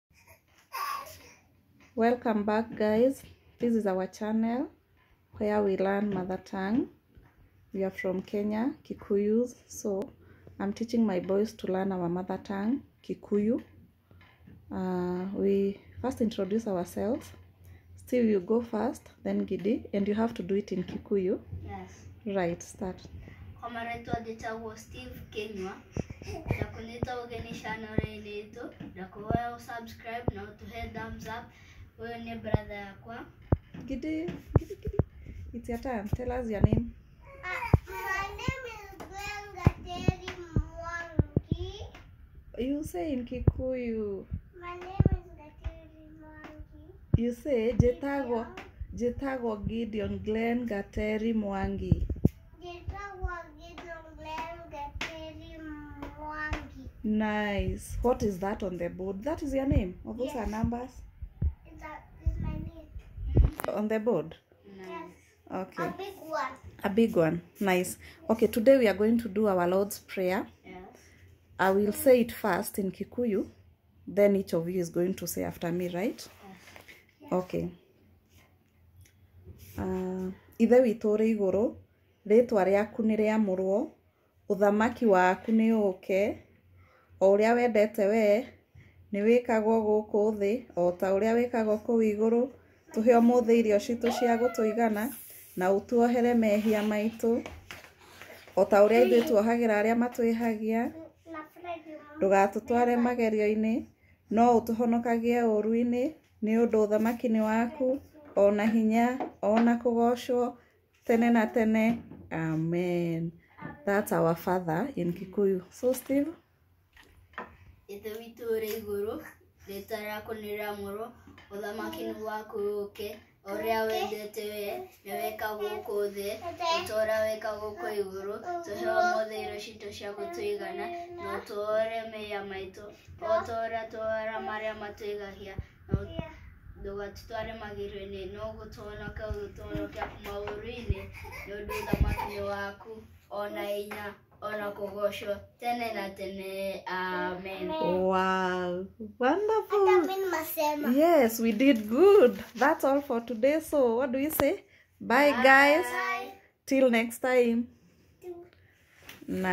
Learning our mother tongue.
Teaching my kids our Lords  prayer in Kikuyu.